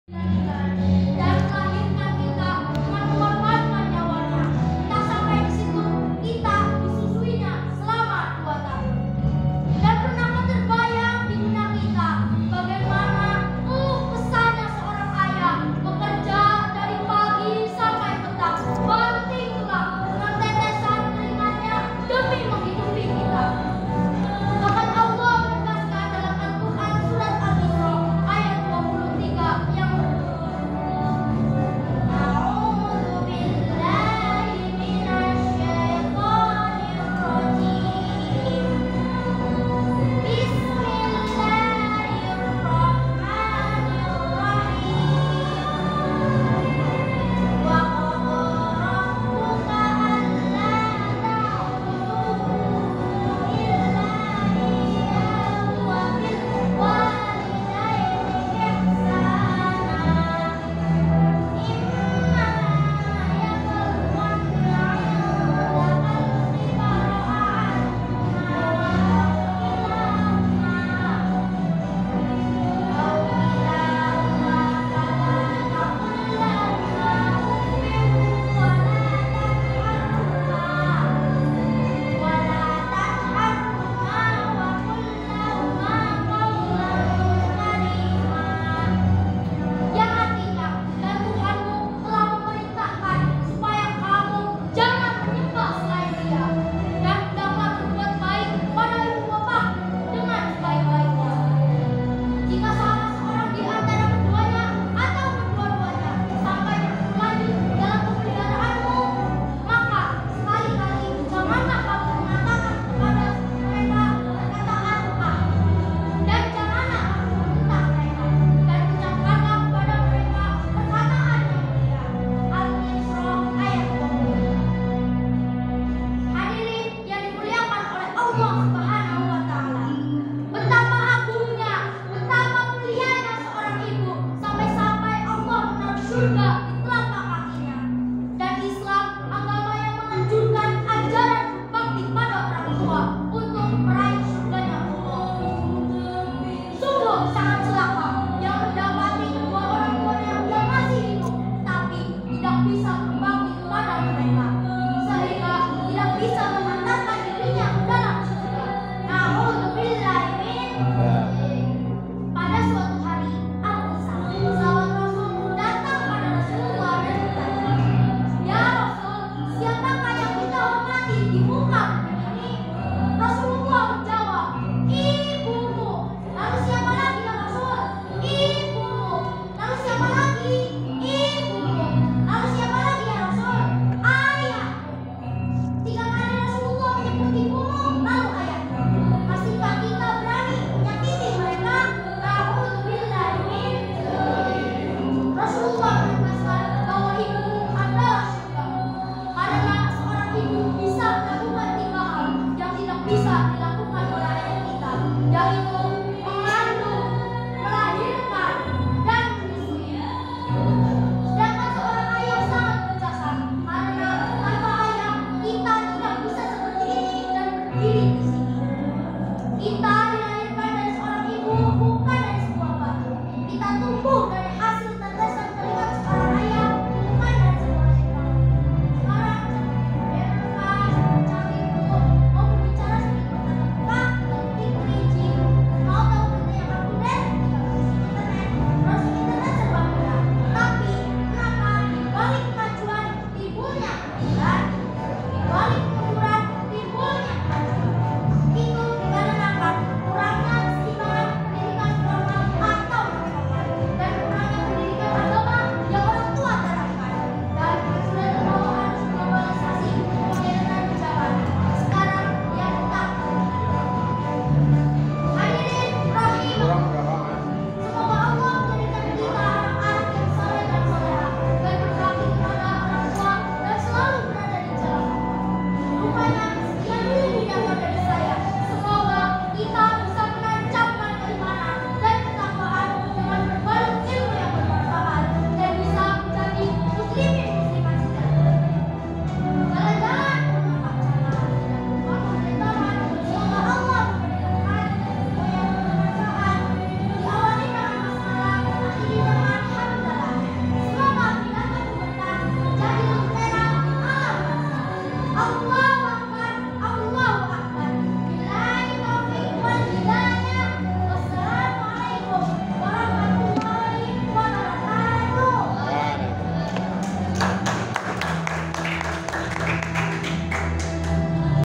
Kultum #19🧶"Bhakti Kepada Kedua Orang Tua" Pildacil Dai Cilik 2023
LOMBA MAPSI SMP KOMDA MAJENANG 2023 DI SMPN 2 MAJENANG 29 Agustus 2023